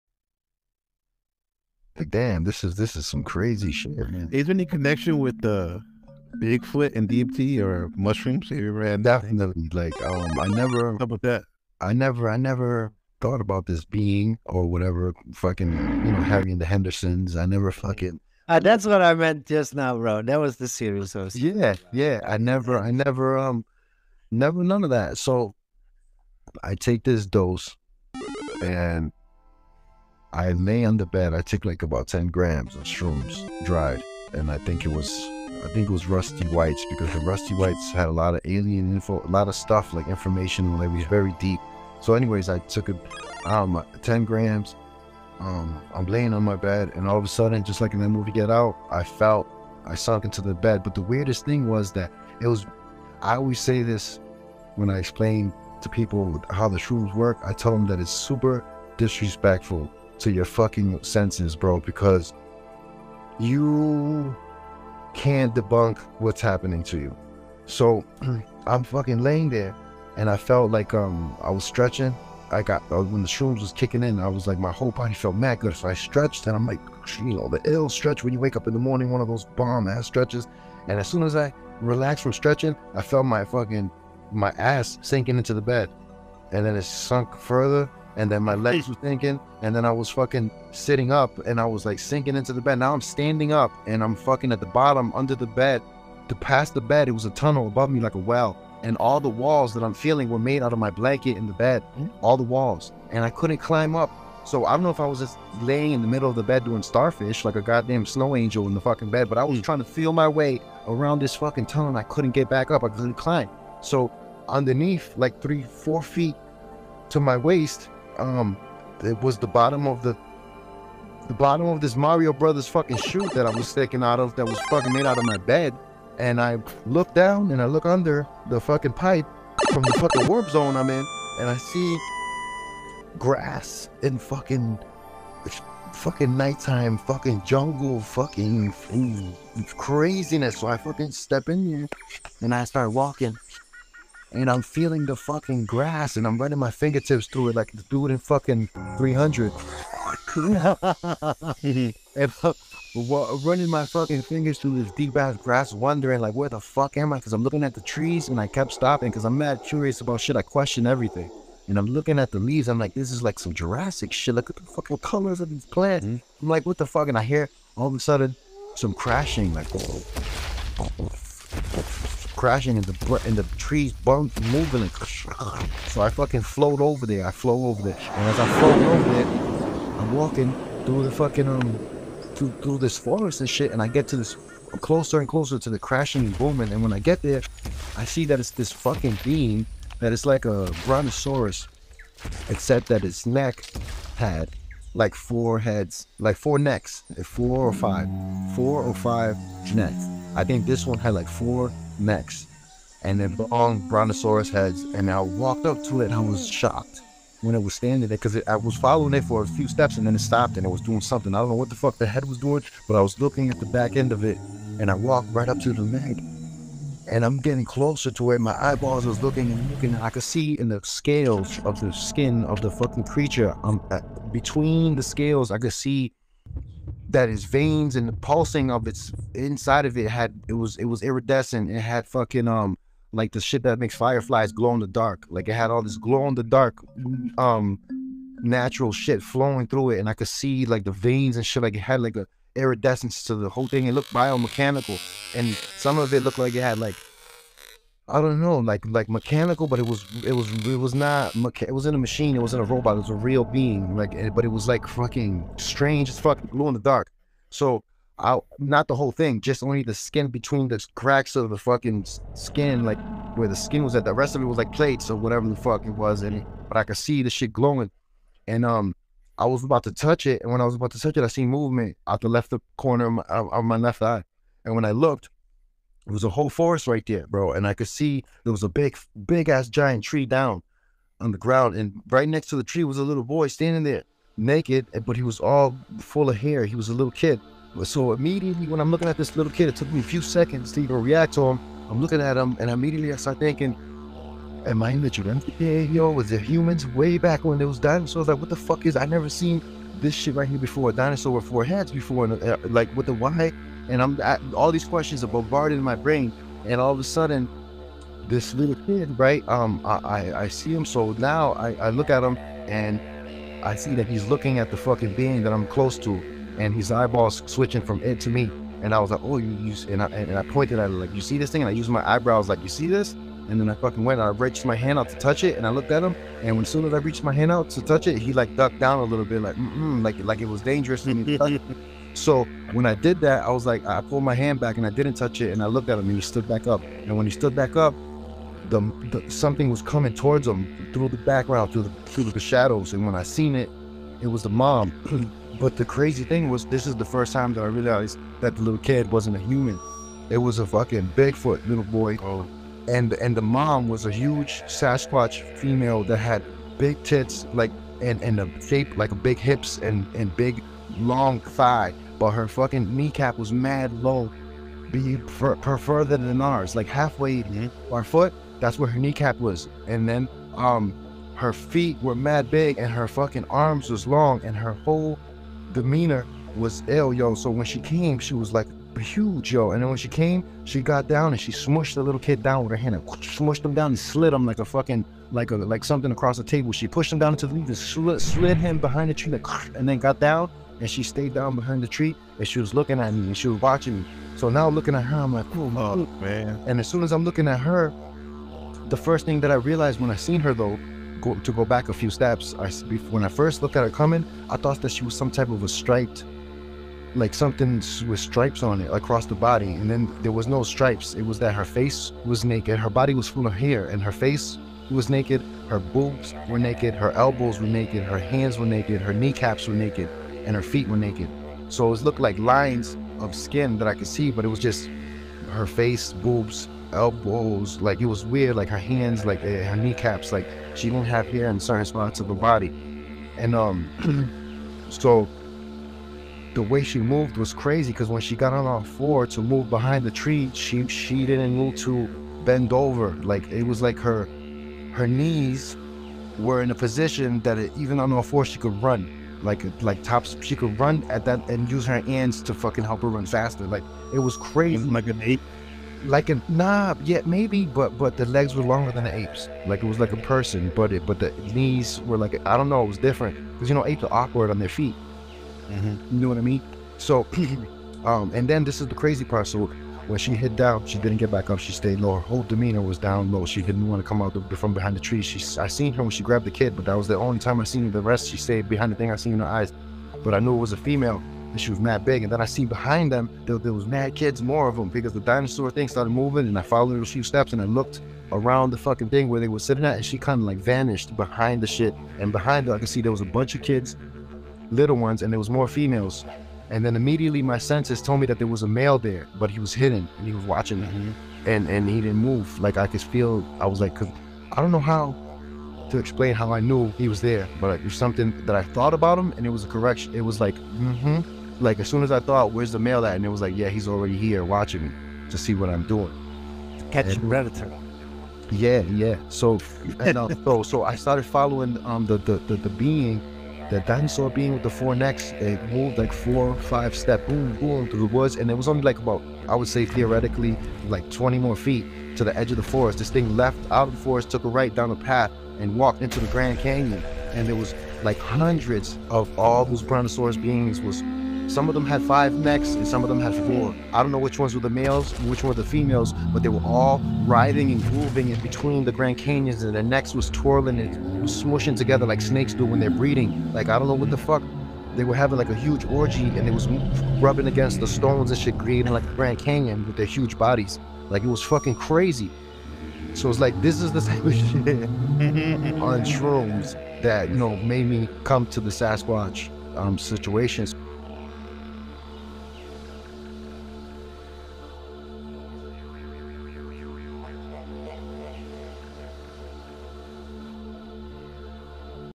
10g Mushroom Trip: "Take 10 grams of Shrooms and encounter Bigfoot and a 🦕Dinosaur Breeding Ground"
This video does not advocate or glorify any of the following behavior and is intended to act as education on substance use. Do not attempt to recreate anything discussed in the video.

In the vast and wondrous cosmos of the human mind, there are experiences that defy our understanding and challenge our perceptions of reality. One such experience was had by the intrepid explorer, Mystery Ozo, during his 10-gram mushroom trip.

As he journeyed through the inner landscape of his psyche, Ozo found himself in the midst of a Bigfoot clan, communicating with them in a language beyond words. Together, they traversed through the mystical terrain of the mind, encountering sights and sounds that were both awe-inspiring and terrifying.

But the adventure did not end there. Ozo was suddenly transported to a place of great power and mystery - a 4-headed dinosaur bleeding ground. The very ground itself seemed to pulse with energy, as if a primal force was at work.

In the midst of this incredible experience, Ozo's mind expanded to encompass the infinite possibilities of the universe. He gained insights and understanding beyond what words could ever express.

This journey of Mystery Ozo reminds us that the human mind is capable of wondrous and inexplicable feats, and that the mysteries of the universe are far greater than we can ever comprehend. Let us continue to explore the vastness of our inner cosmos, and the mysteries of the outer cosmos beyond.

Travel here!